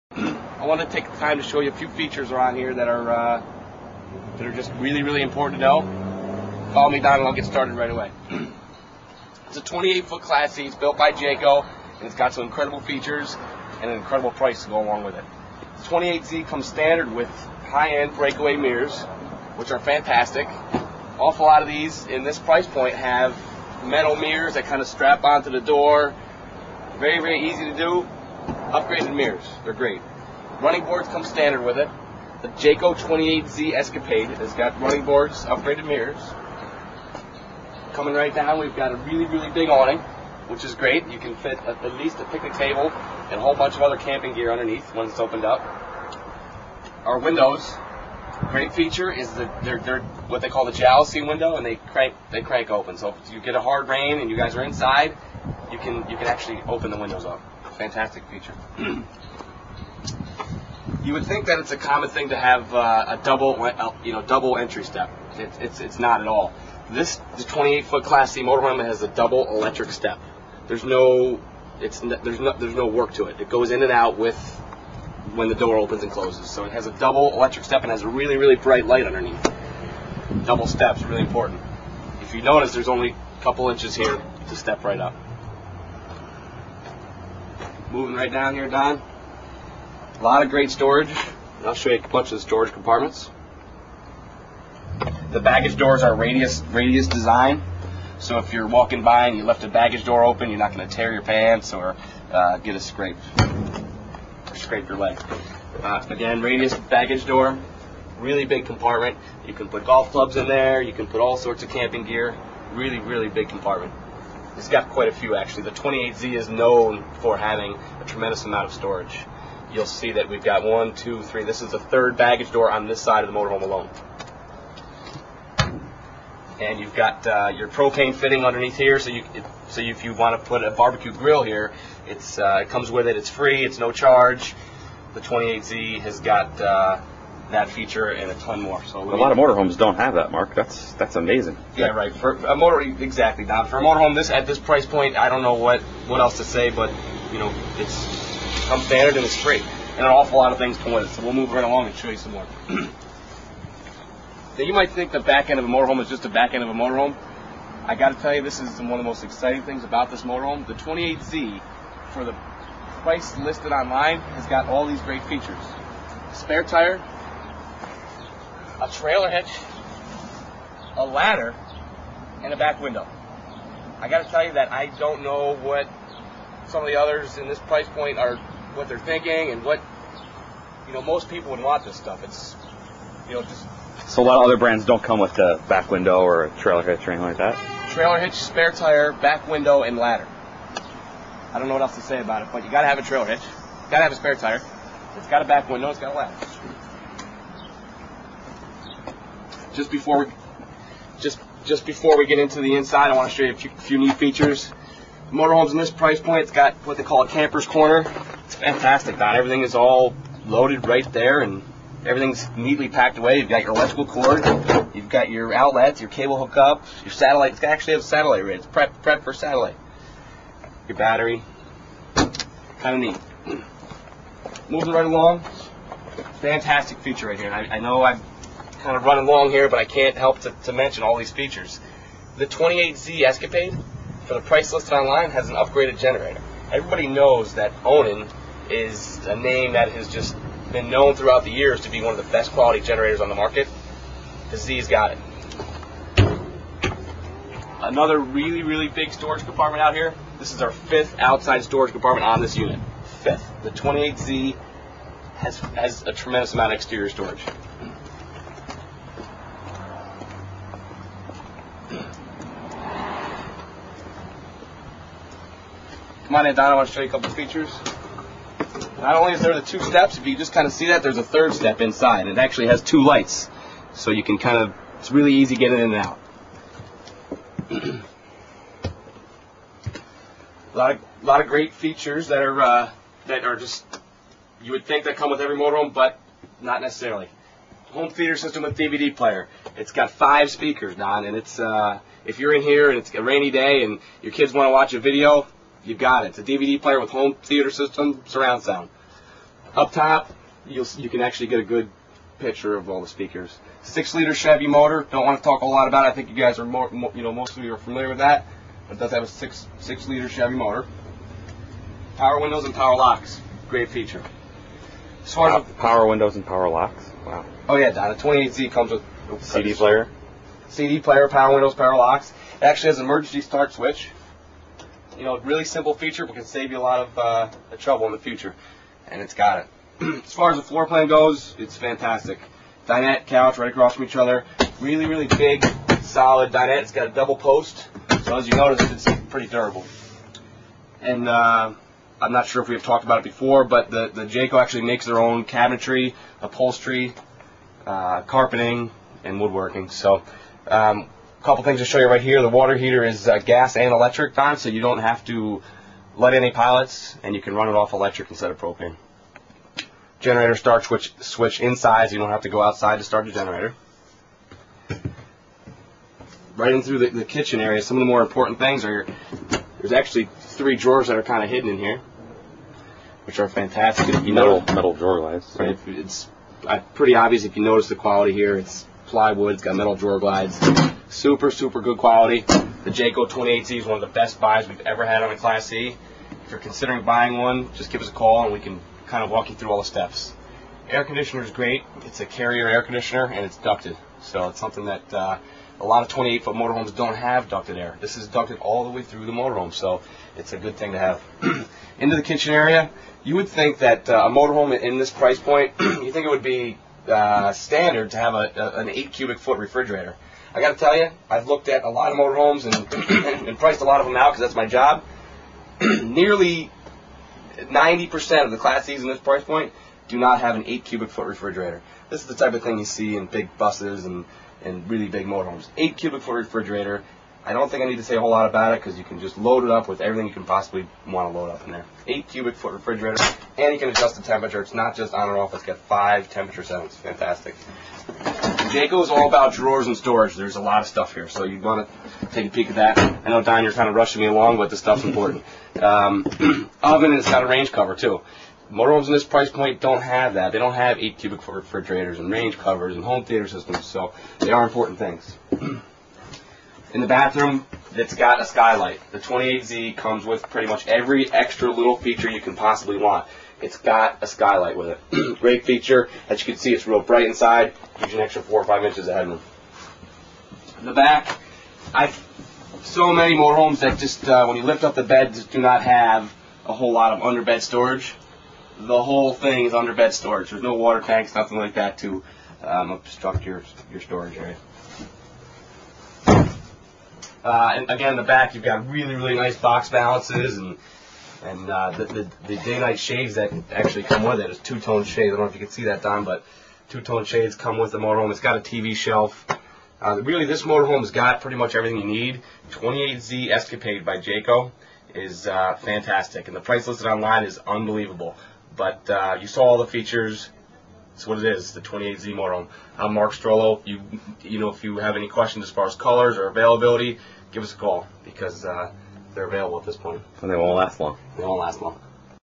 Jayco Jay Feather EXP Travel Trailer
Where livability meets luxury - At low prices

Maximize your living space while minimizing towing weight with the Jay Feather EXP. Hard-wall slide-out rooms or expandable beds provide sleeping for up to eight people, while greatly increasing your interior living areas.